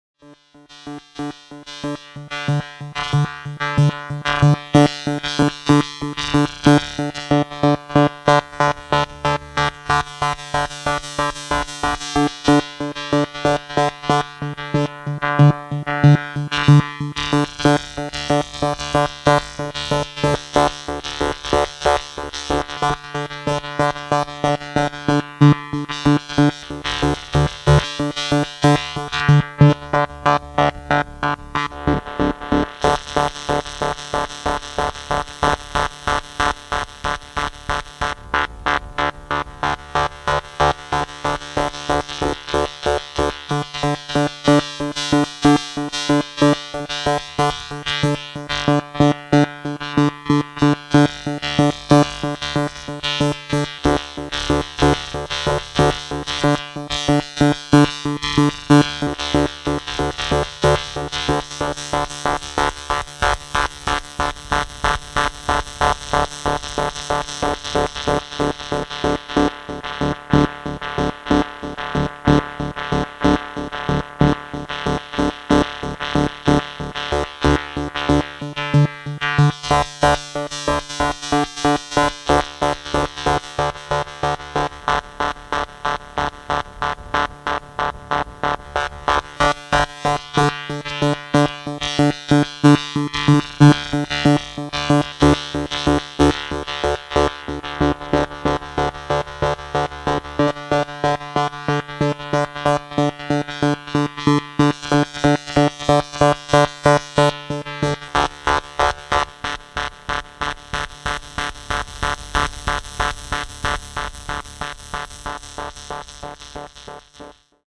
Korg MS-20 self-trigging loop by ESP
self-trigging loop by ESP to get waver sound.
signal from PHONES is converted to trigger by ESP and
another offset filter loop runs by MG is overlapped.